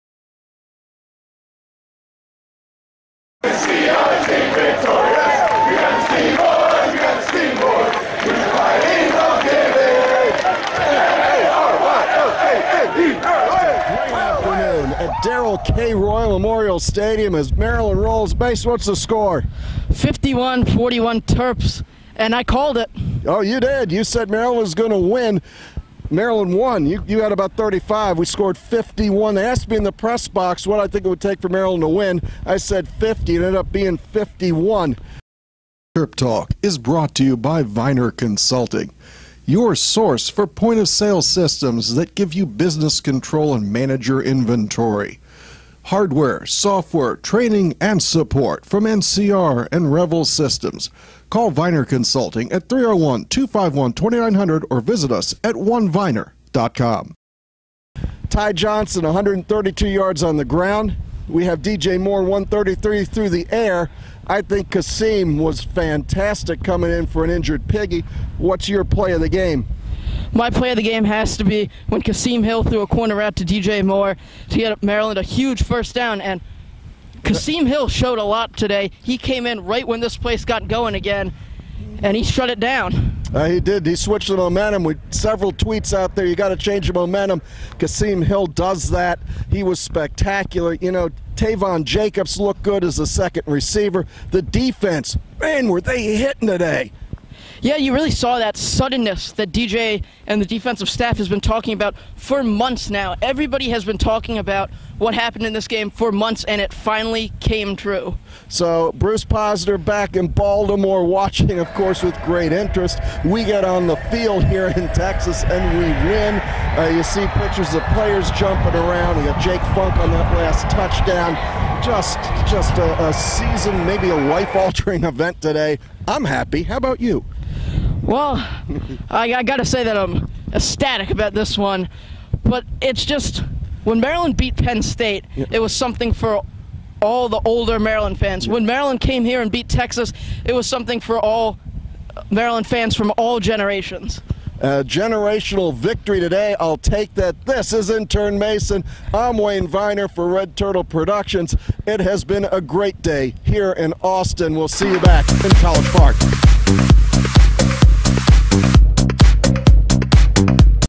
Maryland Football - TerpTalk postgame
Join Wayne Viener and Intern Mason as they discuss the big win from the field in Austin.